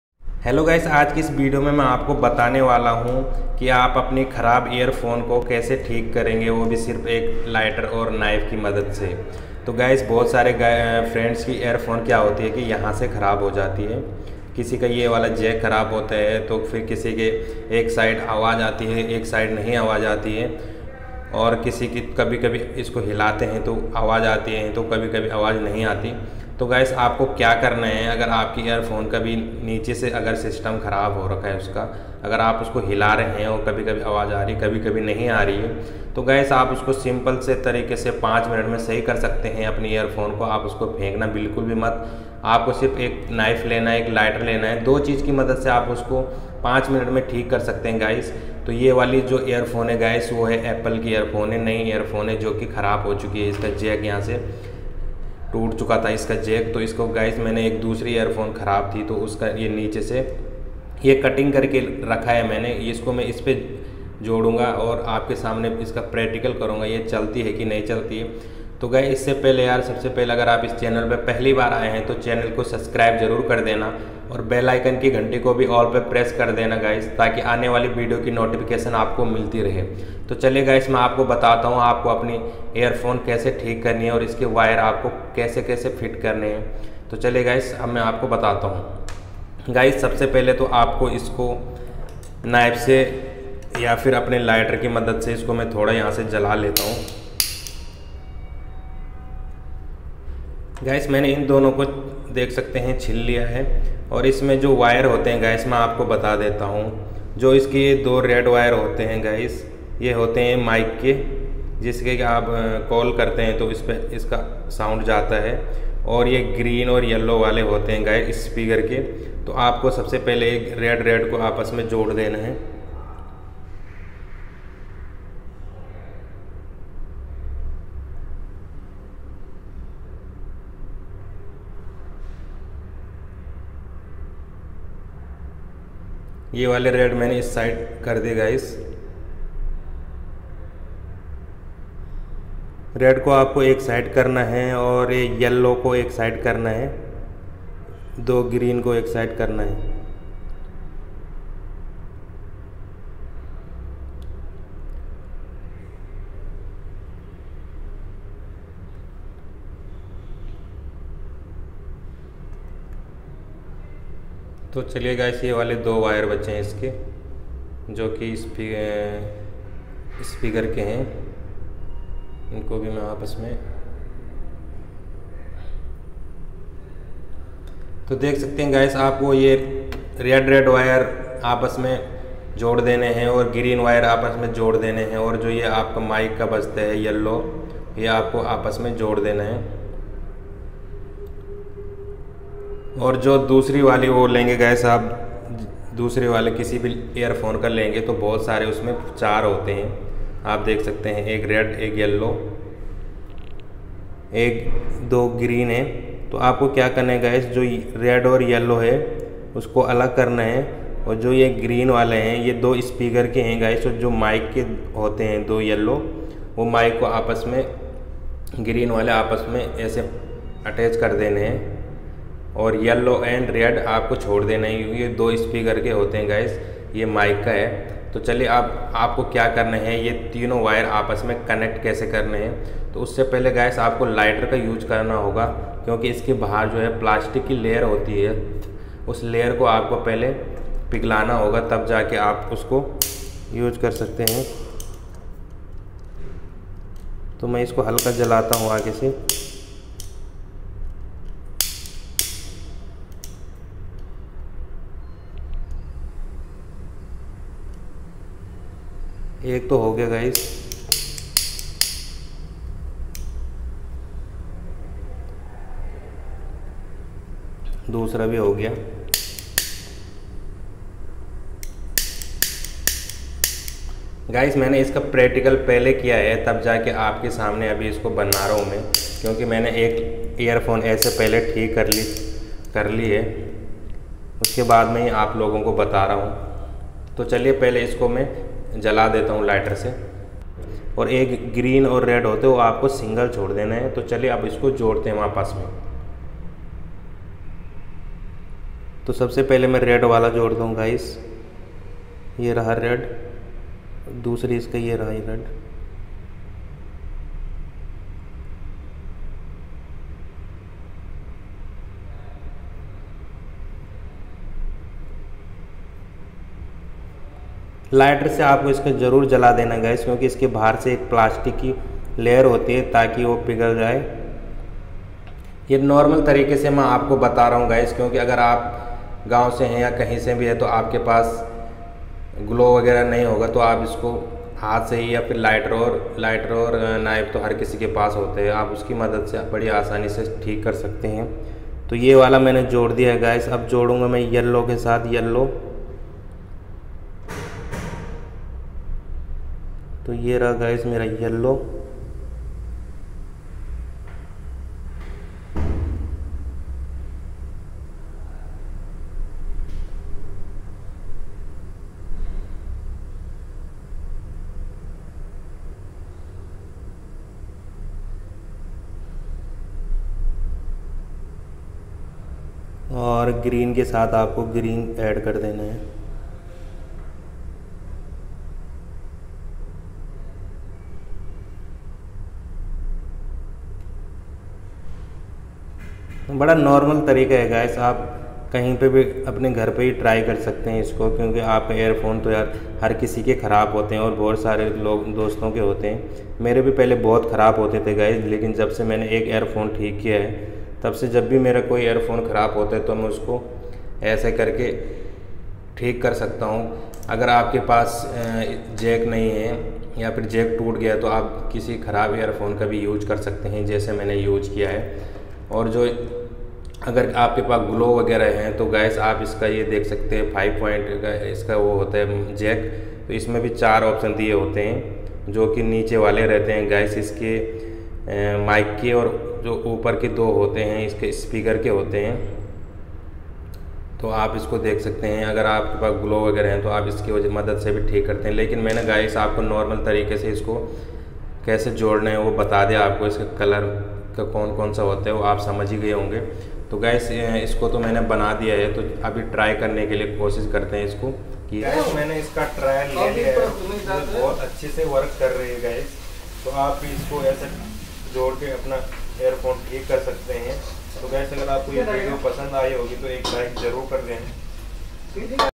How To Repair Earphone || Fix Repair Earphone Jack
Hello friends mera naam hai Pankaj

About this video 👇
Friends aaj ki video main aapko bataunga aap apni kharab earphone ko 5 minute mein kaise theek kar sakte hain lighter aur nife ki madad se